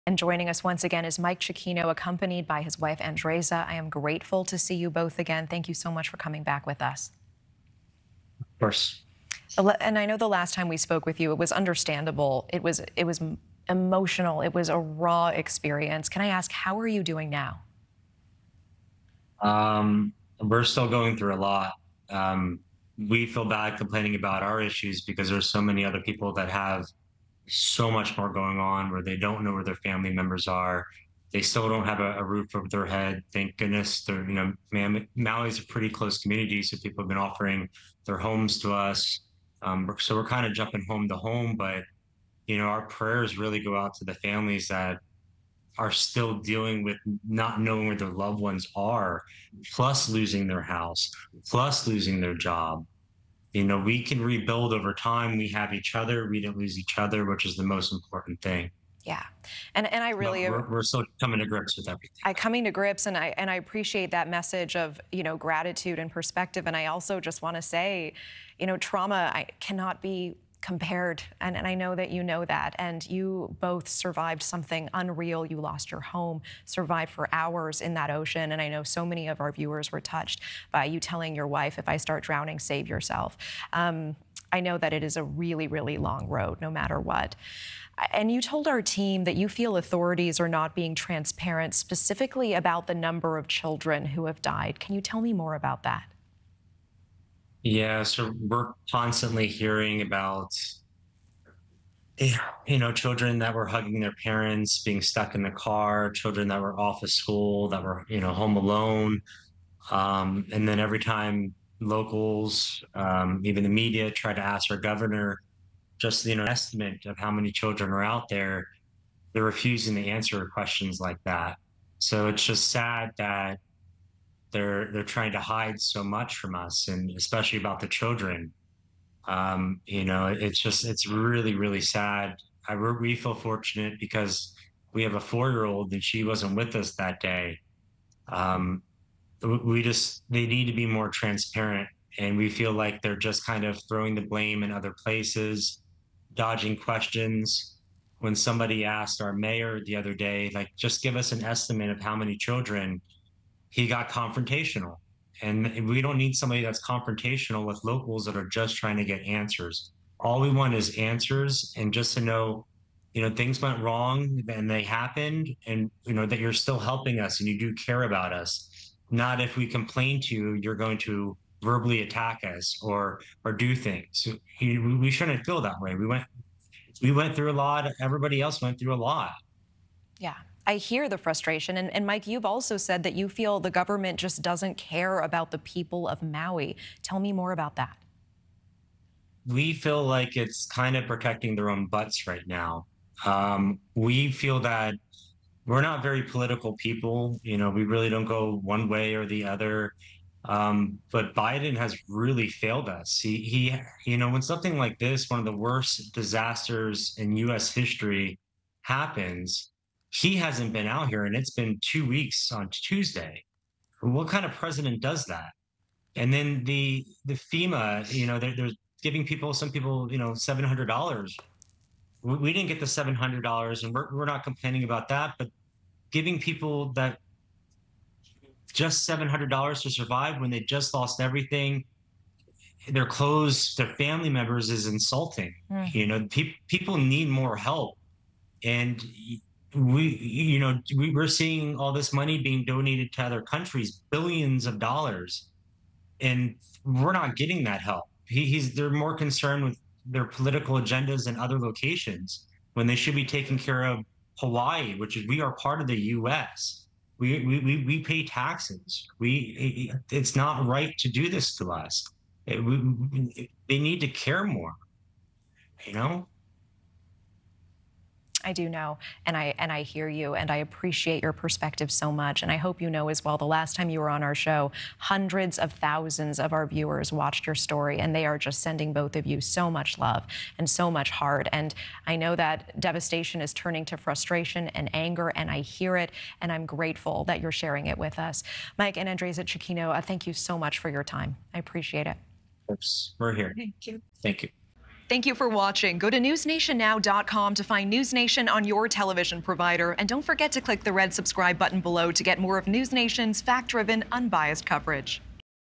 Hawaii resident: ‘Biden has really failed us’ | NewsNation Prime
President Joe Biden is set to visit Maui on Monday. Hawaii resident Mike Cicchino says people need more help and Biden’s response to the wildfires is “not right.”